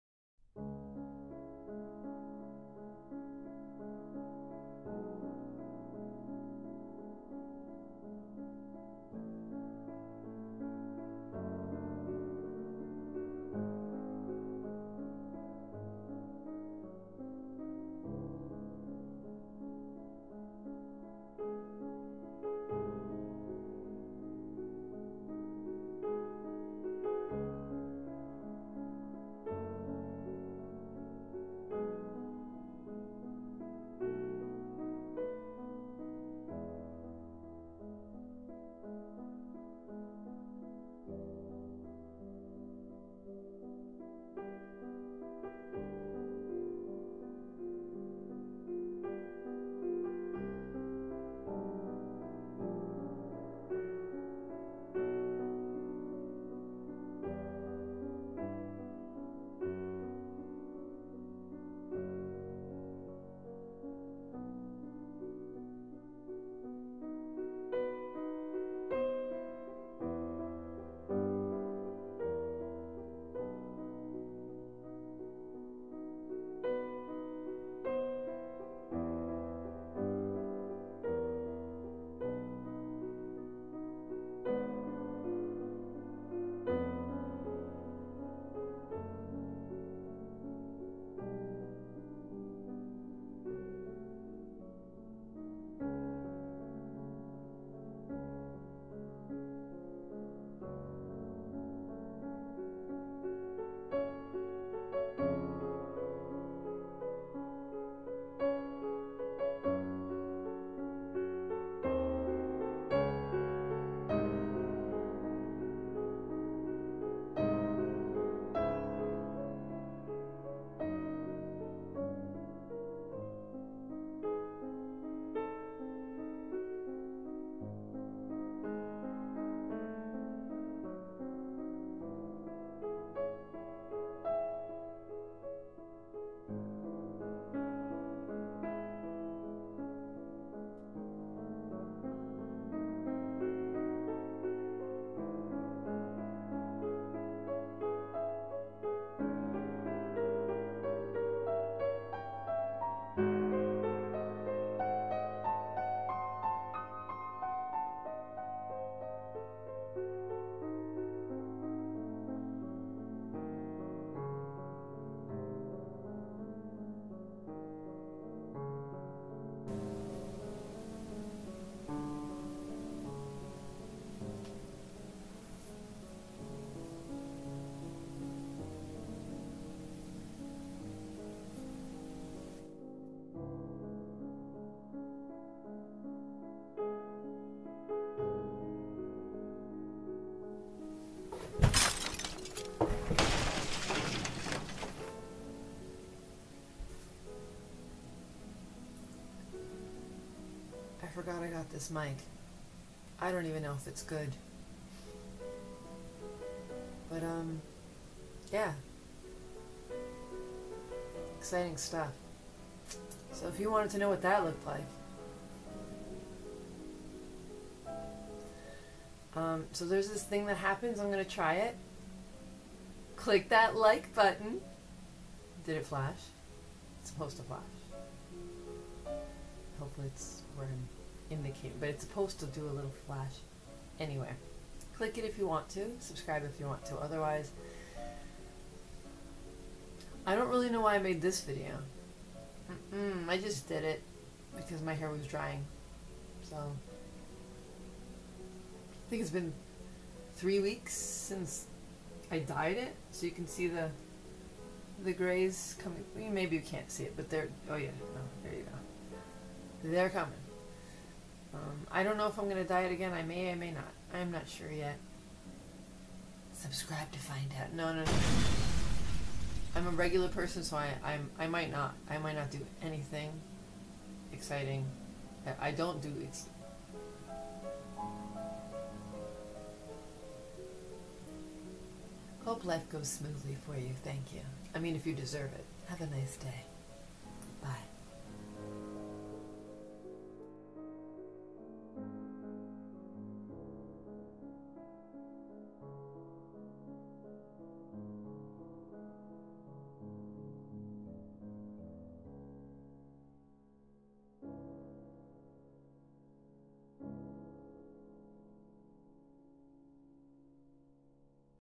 Like watching paint dry but it's hair
I try to be accurate with my titles.

This video was made over a 35 minute period. I didn't just sit there, because I have a son and everything, but I sure tried to make it look like it.

There is no product at all in my hair, obviously. I don't mind the frizz. I think it's funny.

GOOD HEALTH!
Please be good to yourselves and each other.